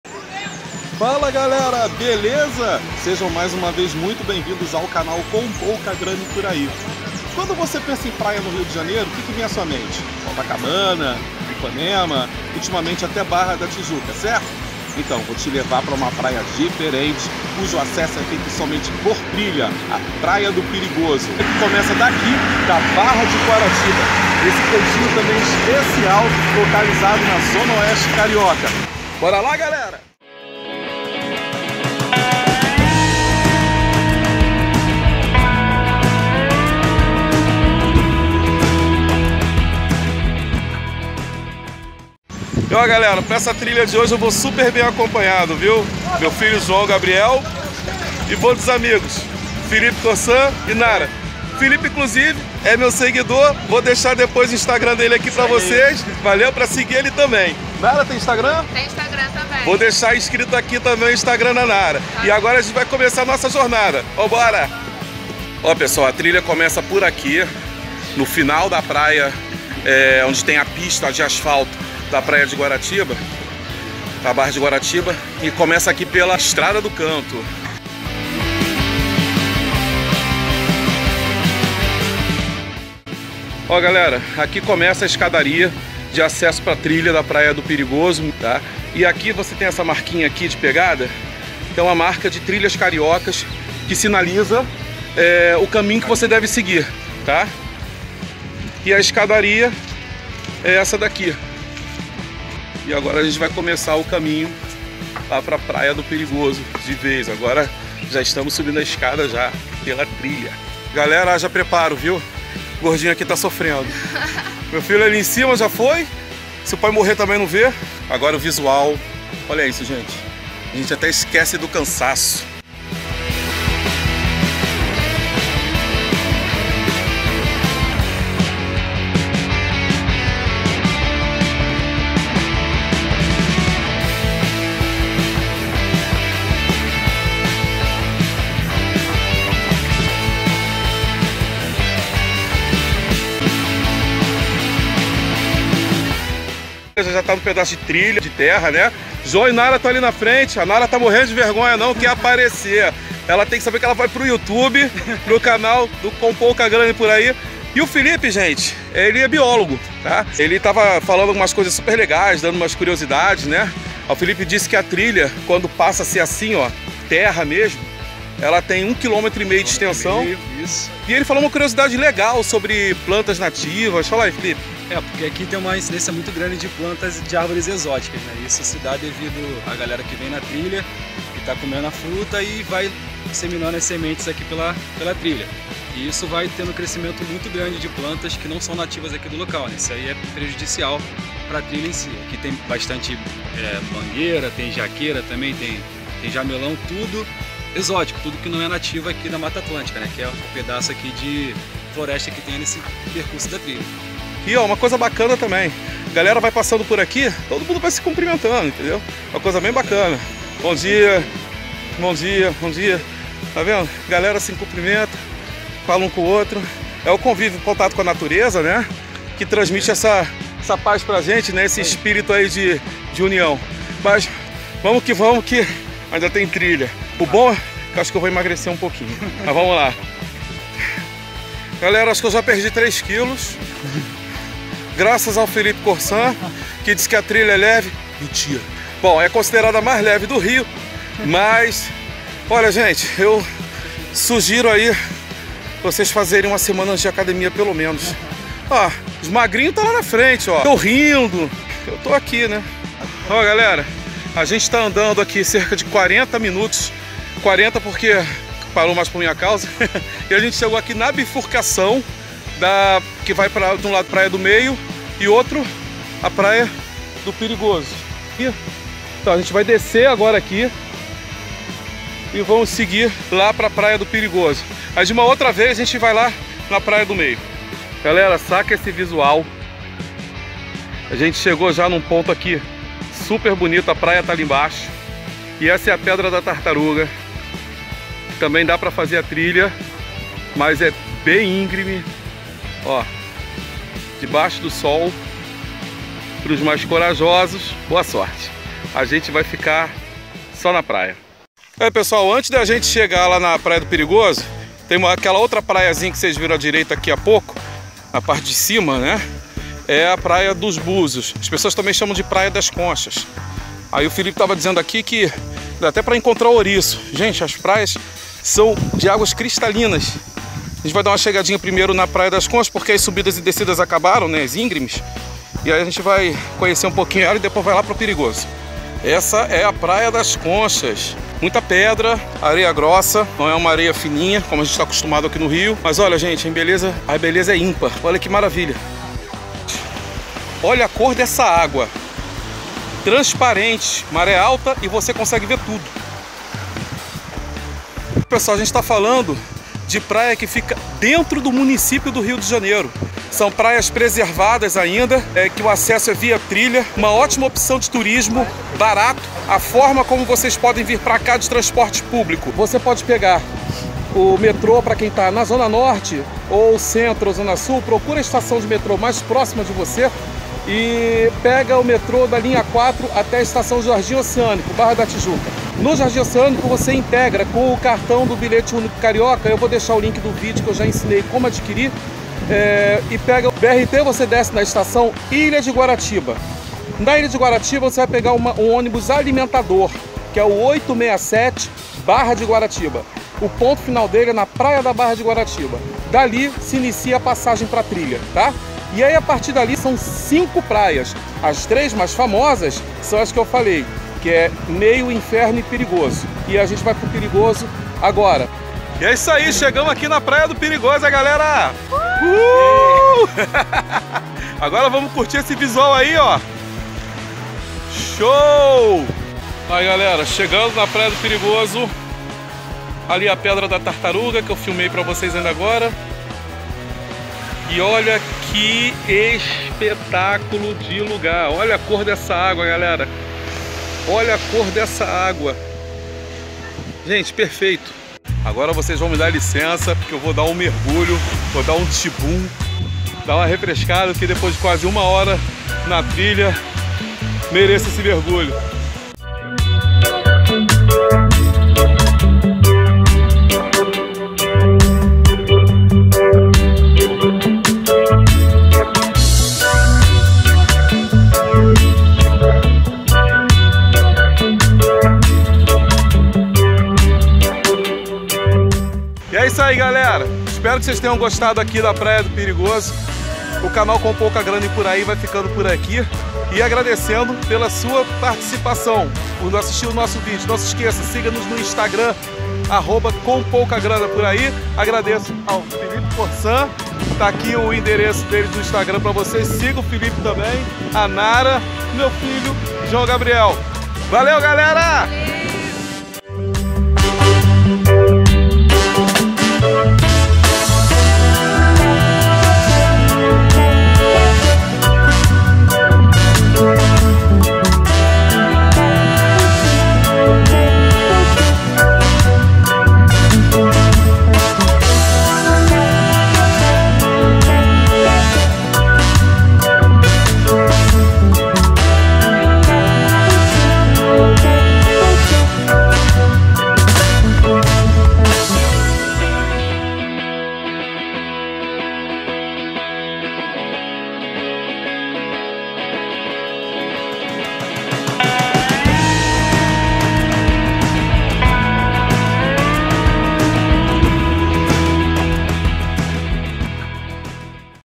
TRILHA PRAIA DO PERIGOSO - RIO DE JANEIRO
EQUIPAMENTOS  E  ÍTENS USADOS NO VÍDEO

Dessa vez, o Canal "Com Pouca Grana E Por Aí..." te leva para a Trilha da Praia do Perigoso, em Guaratiba, Zona Oeste Carioca.

A trilha é considerada leve mas requer disposição! Acompanhe nossa aventura e veja como todo esforço é recompensado, no final, ao chegar a Praia do Perigoso.

Uma correção: na edição, o perfil do Instagram de Nara Oliveira foi informado com equívoco. O correto, é: @soueuanara. Peço desculpas pelo inconveniente.

Um abraço!